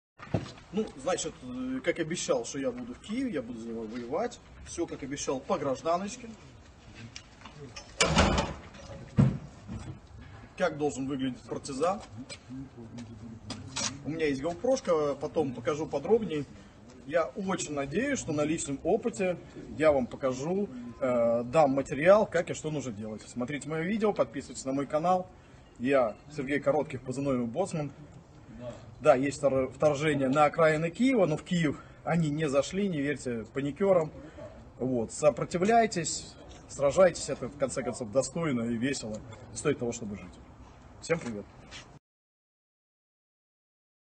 Russia Ukraine War: Urging a guerrilla warfare against Russian Forces
Neo-Nazi Sergey Korotkikh (Malyuta) released a video from Kyiv, urging a guerrilla warfare against Russian Forces.

He seems to he using an AR-15 carbine (~10.5") with possible EOTech HHS III (featuring G33.STS magnifier & EOTech 518-2 sight).

Video by: @war_noir (Twitter)
Part-time Weapons and Conflicts researcher | I focus on Non-State Armed Groups & Illicit Weapons Sales | Donate | ✍️ @MilitantWire